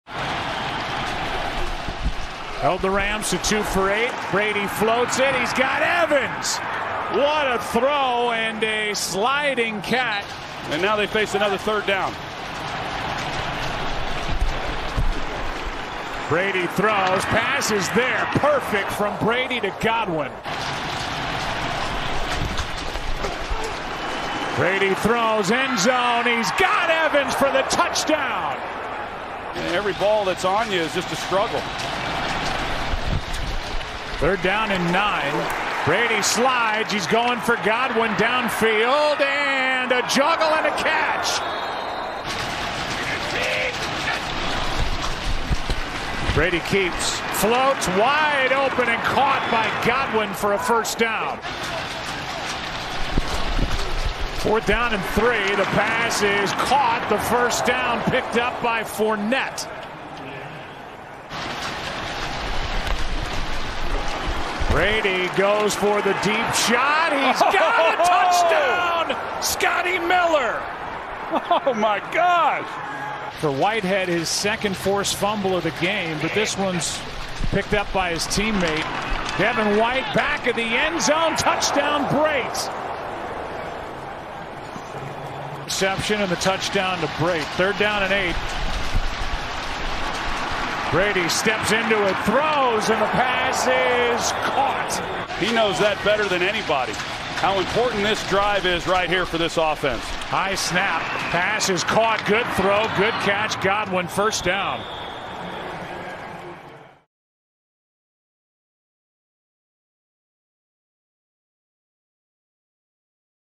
Tom Brady the GOAT Let's Rip w/ 3 TDs to Go to the Super Bowl Once More | NFL 2020 Highlights
Tom Brady fires away with 280 passing yards and 3 touchdowns to win the AFC Championship and send the Buccaneers to the Super Bowl. The Tampa Bay Buccaneers take on the Green Bay Packers during the NFC Championship Game of the 2020 NFL postseason.